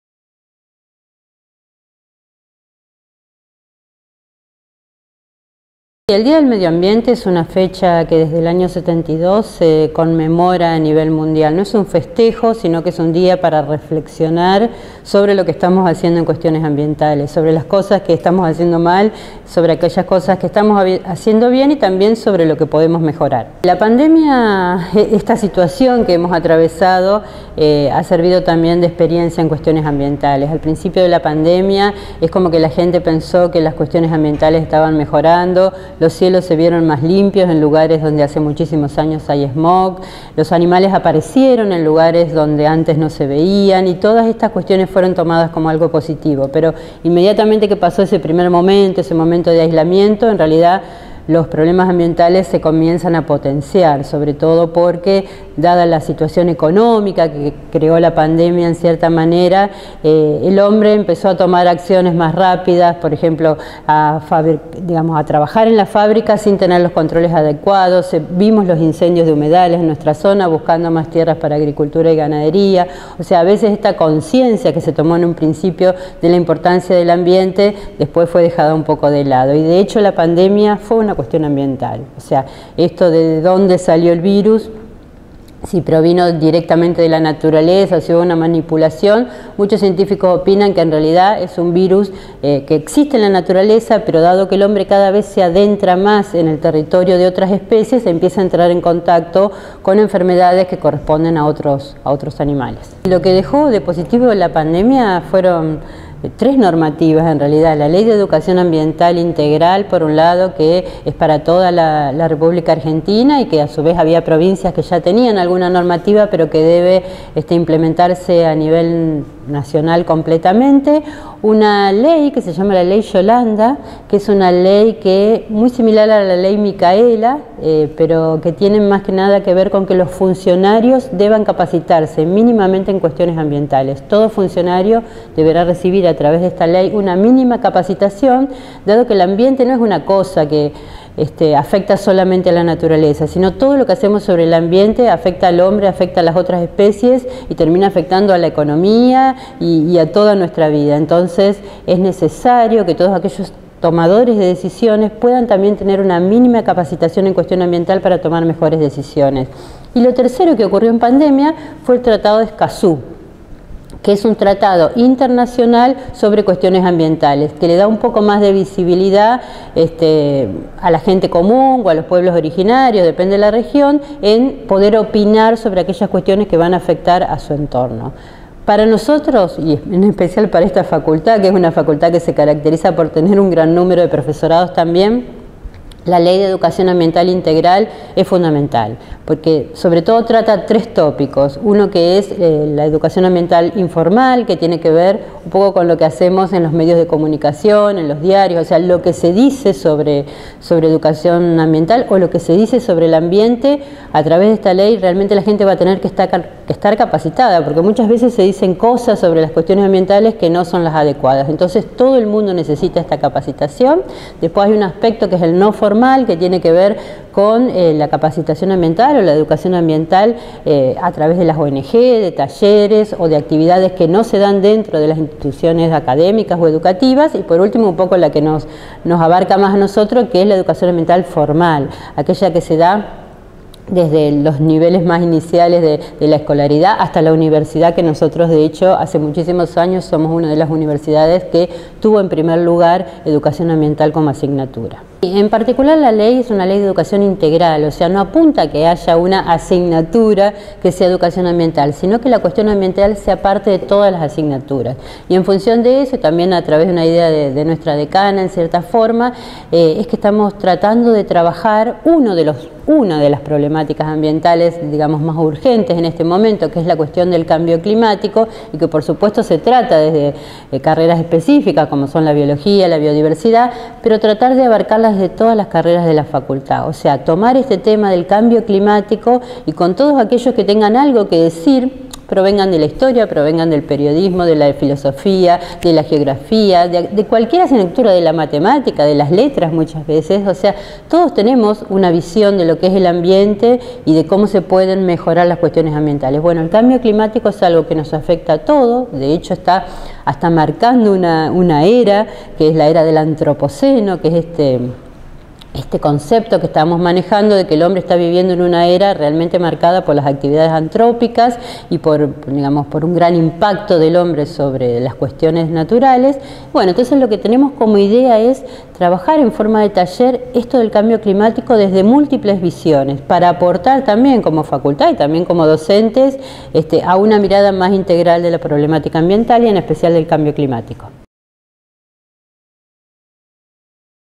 Medioambiente y cambio climático: Alba Imhof - Docente FHUC UNL
El 5 de junio se conmemoró el Día Mundial del Medio Ambiente.

La Prof. Alba Imhof (responsable de la cátedra "Educación ambiental" de la FHUC), realiza una reflexión al respecto, analizando el impacto de la pandemia en la problemática, y los desafíos que se presentan a corto y mediano plazo.

Además, brinda detalles de la convocatoria “Cambio climático: diálogo entre disciplinas”, abierta a docentes e investigadores/as de las distintas carreras de la Facultad, para compartir miradas e intercambiar saberes entre disciplinas, tomando como eje de reflexión el cambio climático, en el marco de la implementación de la Ley de Educación Ambiental.